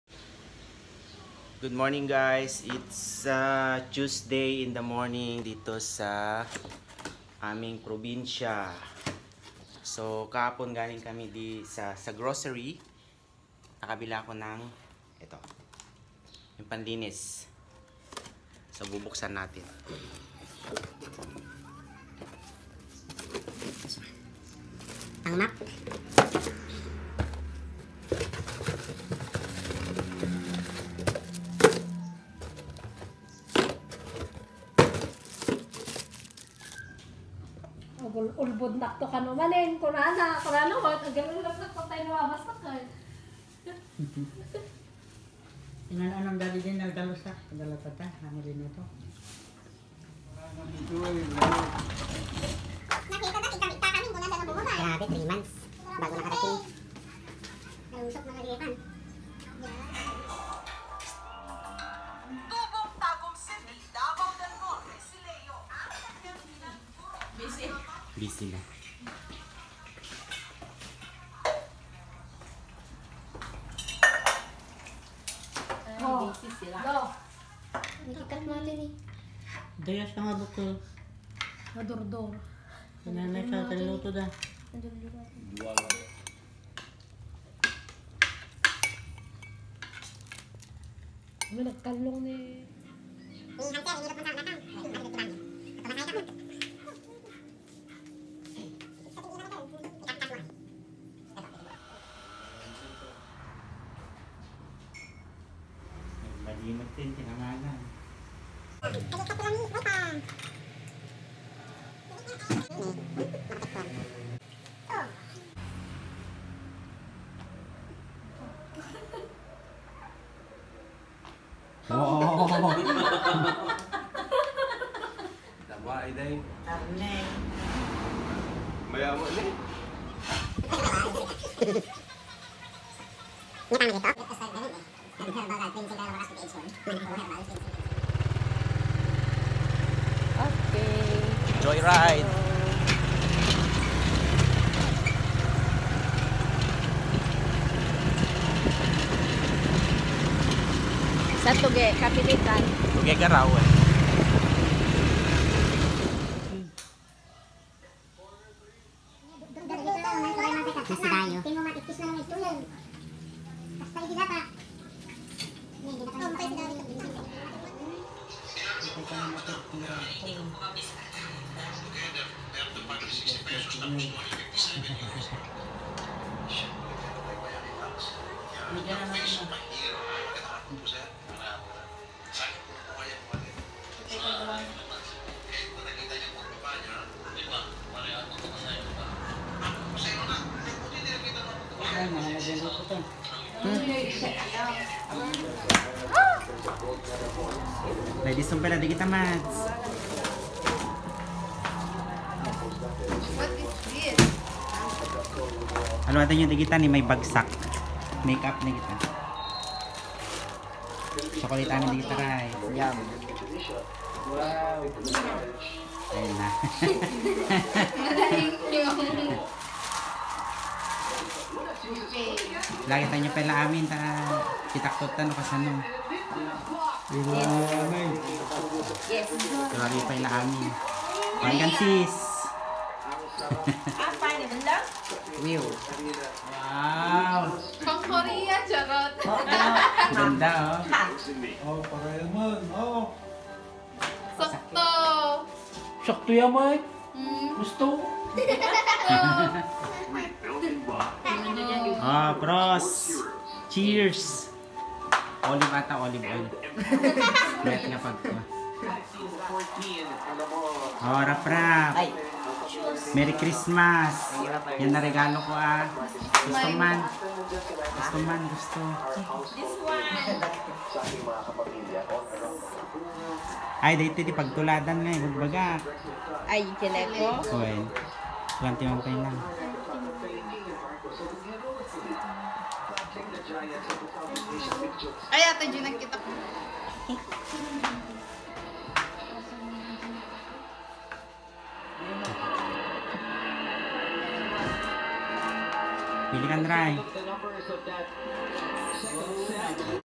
My Box Has Finally Arrived! || VLOG #95
After more than 3.5 months than the normal 1.5 months of waiting, my package have been safely delivered. I was little nervous, because  I put all my presents in there for
my family. Thankfully everything’s are completely no damage.